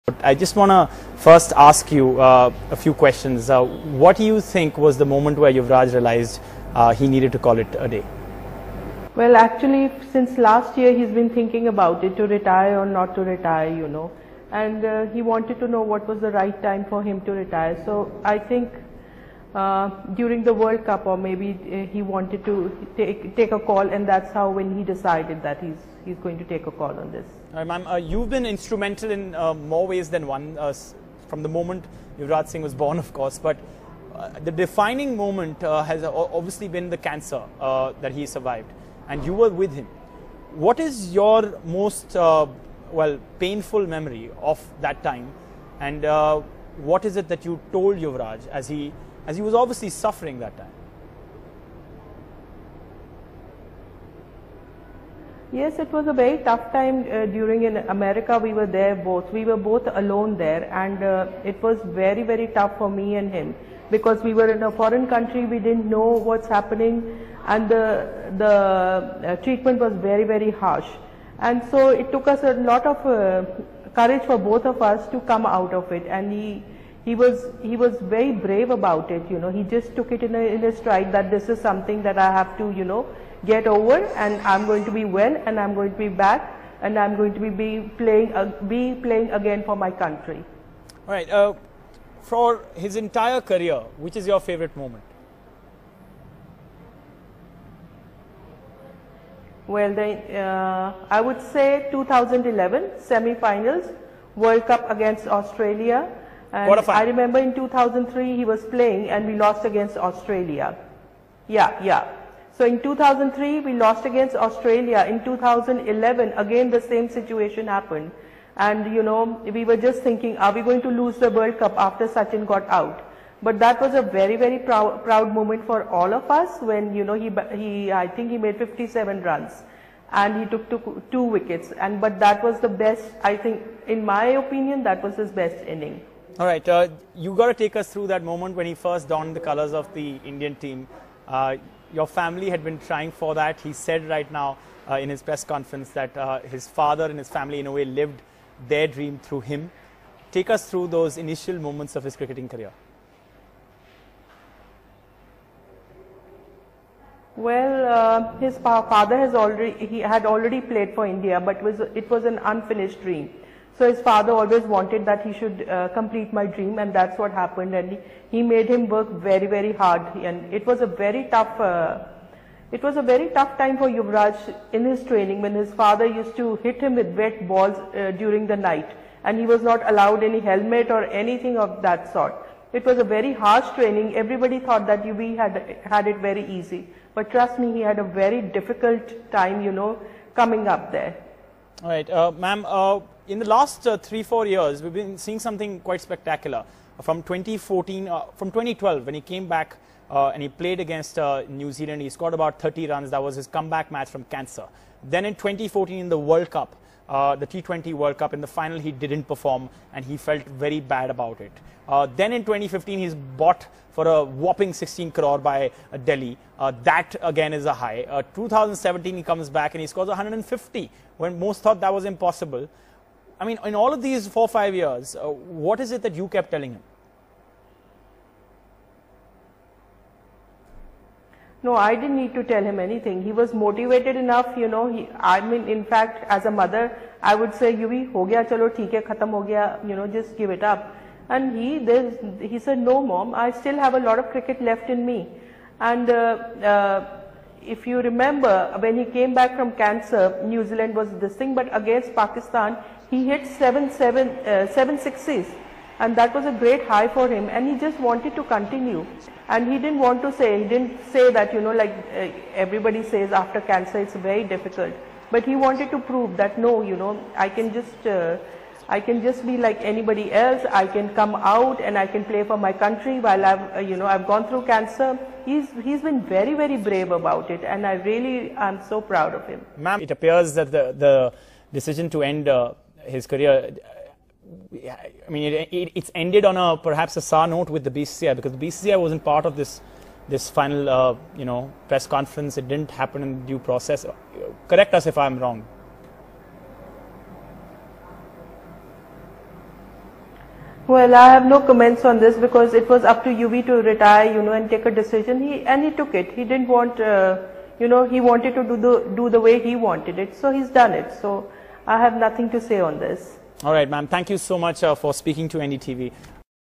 Yuvraj Singh's Comeback From Cancer Was Extremely Tough, Says Mother Shabnam
NDTV speaks exclusively to Yuvraj Singh's mother, Shabnam. She says it was extremely tough for Yuvi to comeback from cancer but refuses to comment on the relationship between Yuvraj and the BCCI at the end of his career. She says the decision to retire was his.